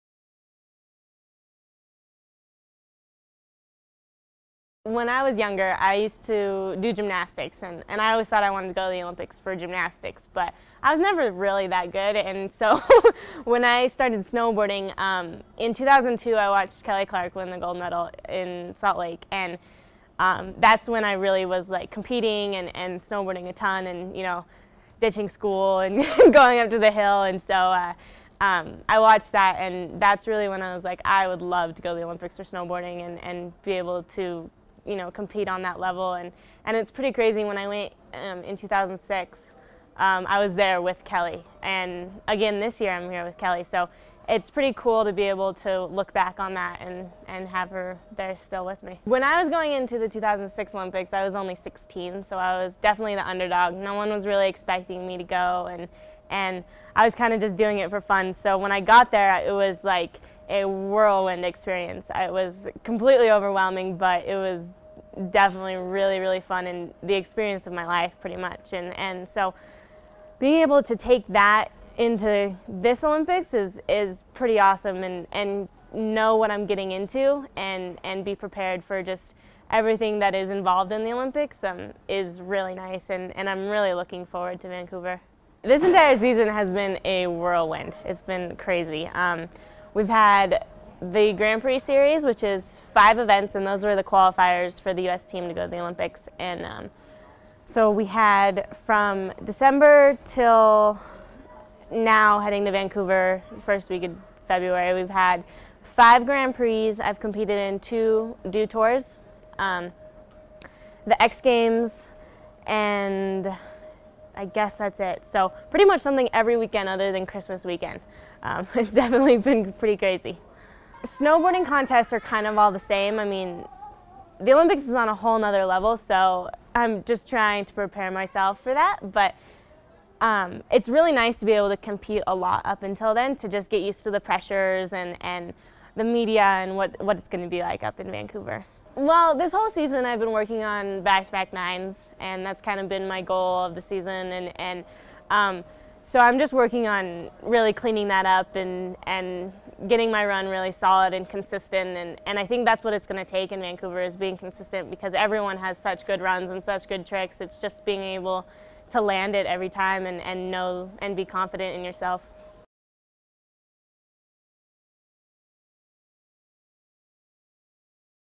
Tahoe TV Talks To Elena Hight
Tahoe TV caught up with US Olympic Snowboarder Elena Hight at Northstar-At-Tahoe before she headed to Vancouver.